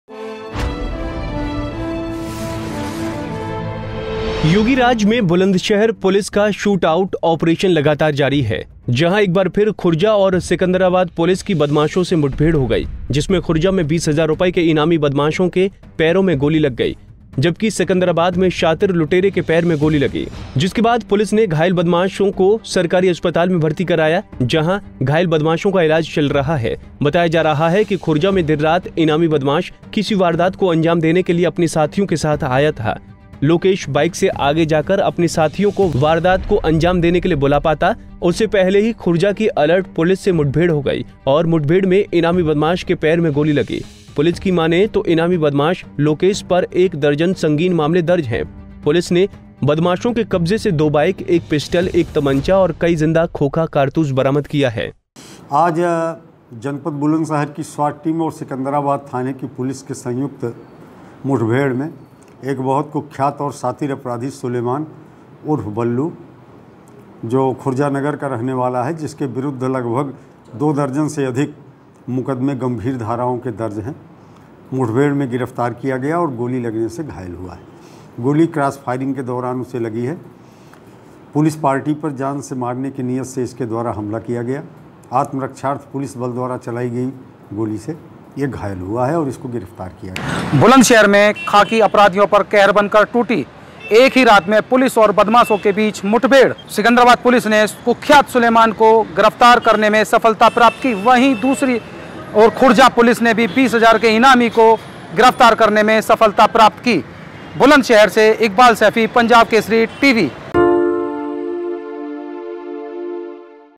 बुलंदशहर पुलिस का शूट आउट ऑपरेशन जारी, अलग-2 जगह दो इनामी बदमाश दबोचे
योगीराज में बुलंदशहर पुलिस का शूट आउट ऑपरेशन लगातार जारी है...जहां एक बार फिर खुर्जा और सिकंदराबाद पुलिस की बदमाशों से मुठभेड़ हो गई...जिसमें खुर्जा में 20 हजार रूपए के इनामी बदमाशों के पैरों में गोली लग गई...जबकि सिकंदराबाद में शातिर लुटेरे के पैर में गोली लगी...जिसके बाद पुलिस ने घायल बदमाशों को सरकारी अस्पताल में भर्ती कराया...जहां घायल बदमाशों का इलाज चल रहा है...